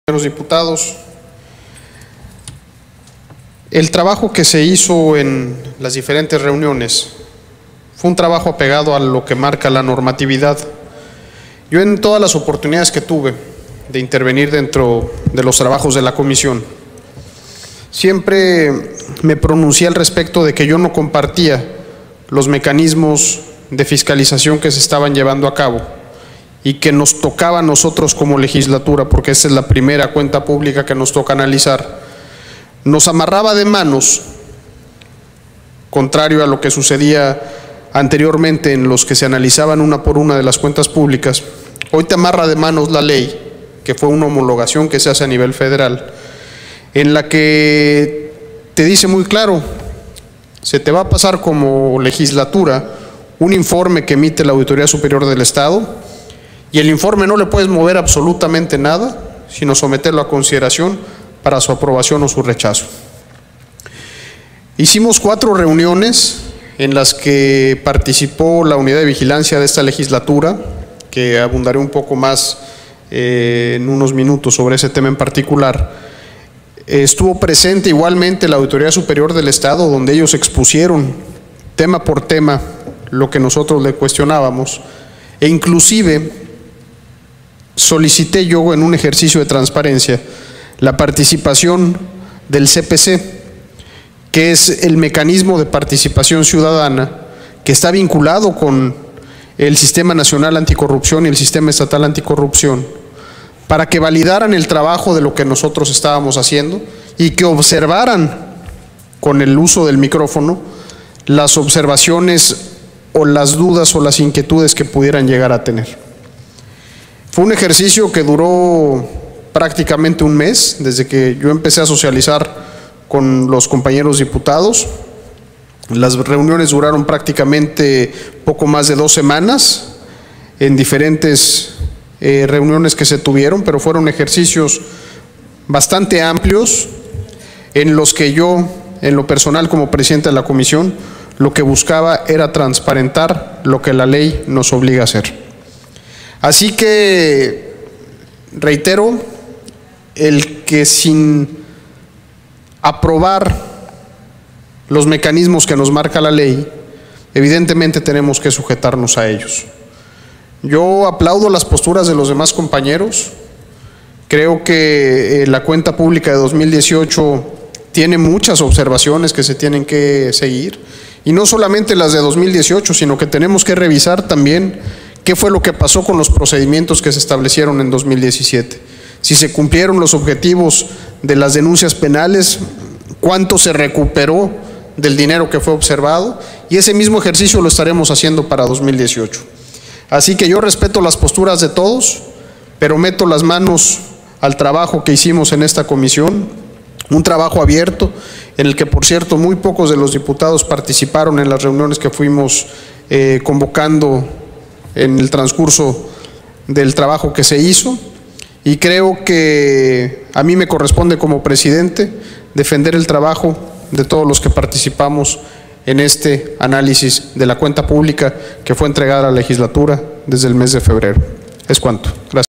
Intervención del Dip. José de la Peña Ruiz de Chávez. PVEM (29/Oct/2020)
Intervención del Diputado José de la Peña Ruiz de Chávez, del PVEM, en relación al Dictamen con Minuta de Decreto por el que la Honorable XVI Legislatura del Estado Libre y Soberano de Quintana Roo, aprueba el informe general ejecutivo relativo a la fiscalización superior de la cuenta pública para el ejercicio fiscal 2018 y emite las conclusiones y recomendaciones relativas a dicha fiscalización